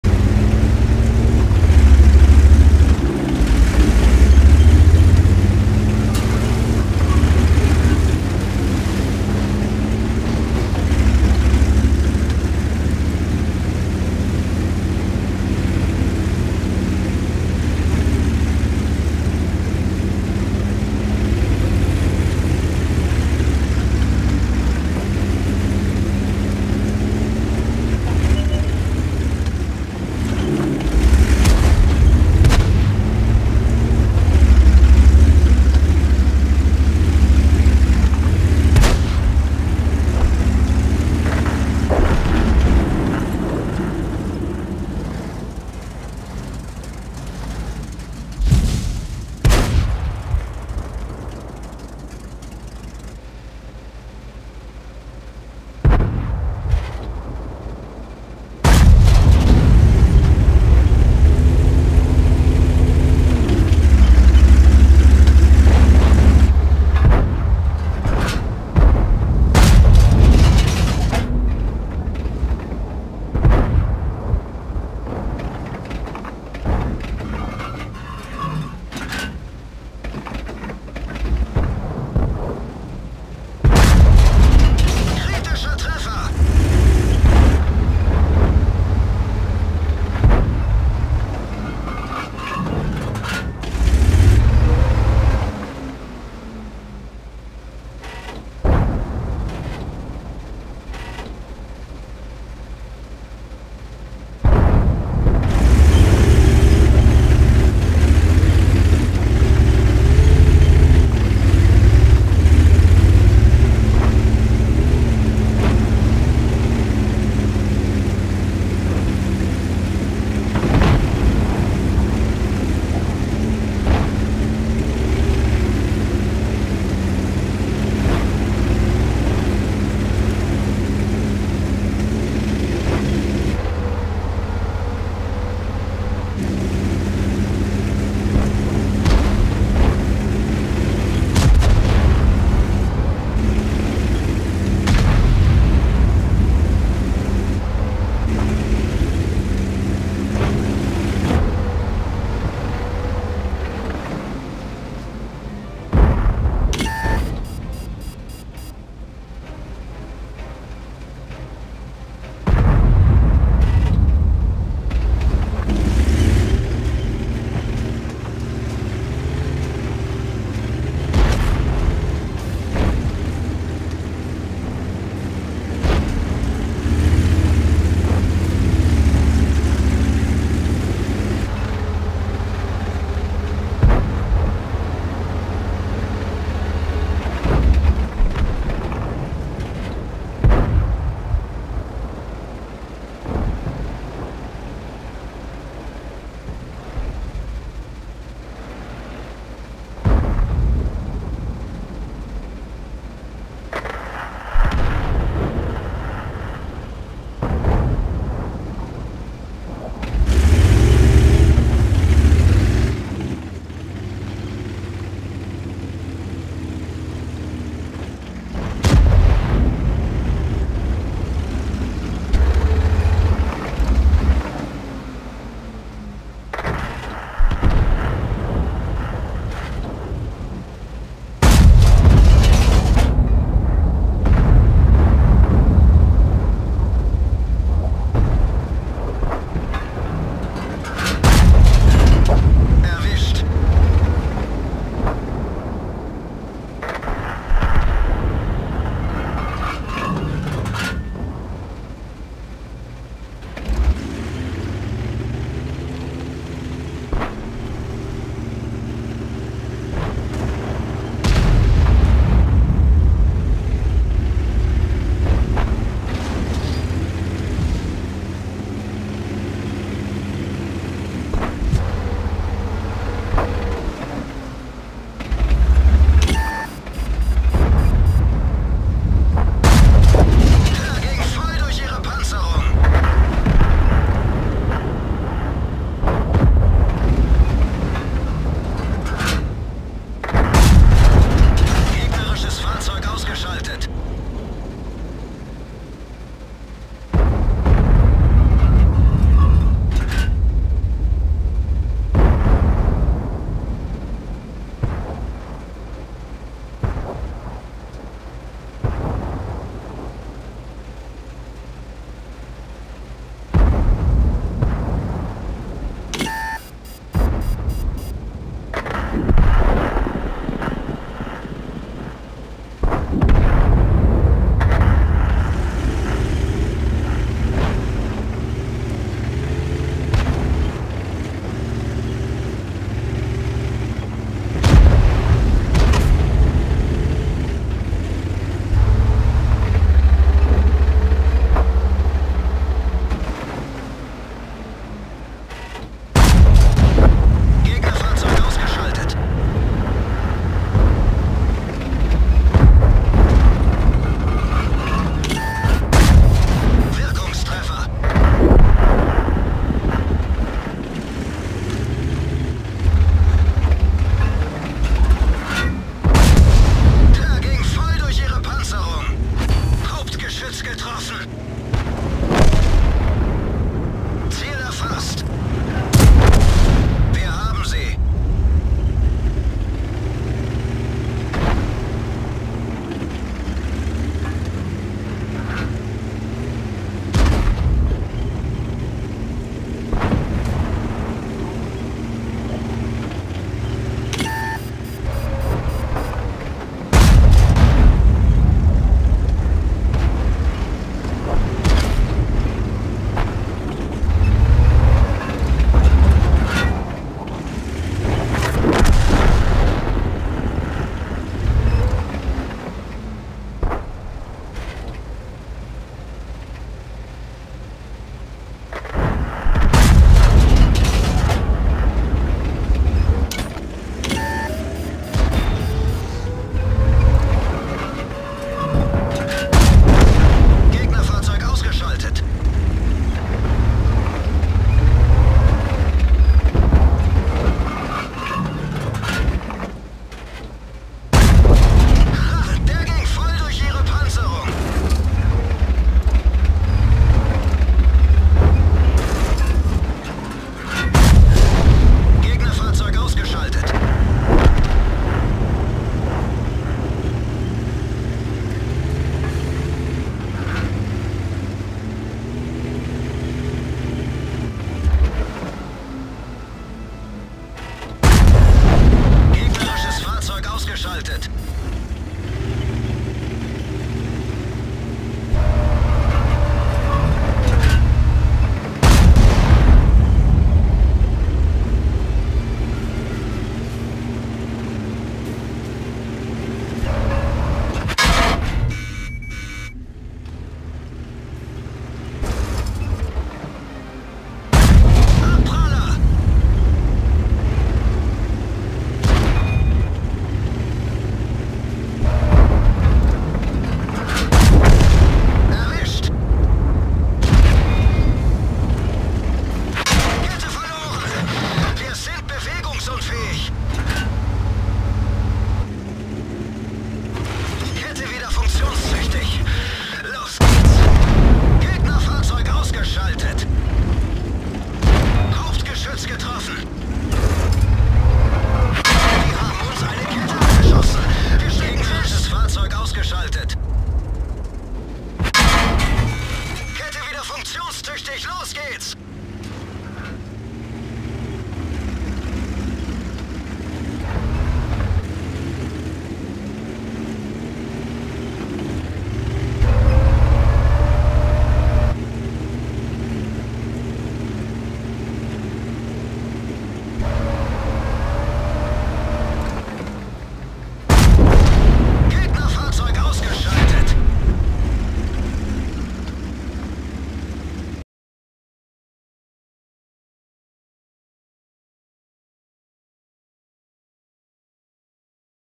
11 Фрагов на яге
Игрок: Sir_T4 [IS__]
11 Фрагов на яге

Ну не хочет команда  выигрывать.